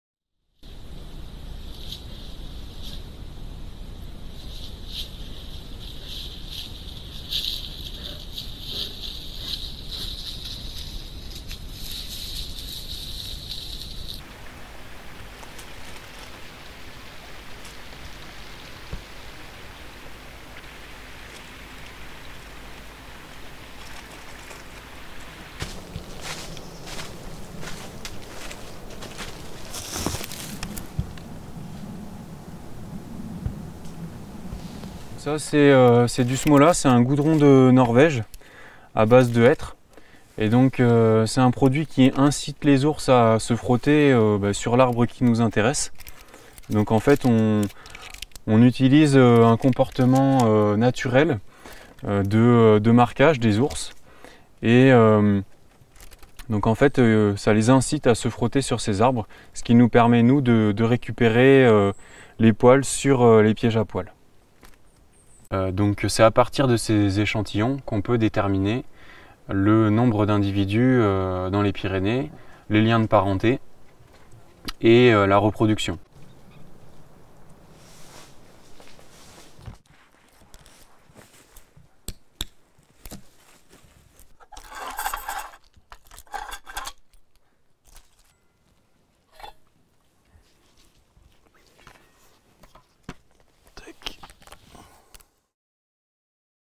Experts monitor brown bear population in Pyrenees | AFP
In the Pyrenees, 70 brown bears roam freely, under the monitoring of experts who, surveying steep forests, track down clues confirming the presence of the protected plantigrade, after having almost disappeared from these mountains between France and Spain.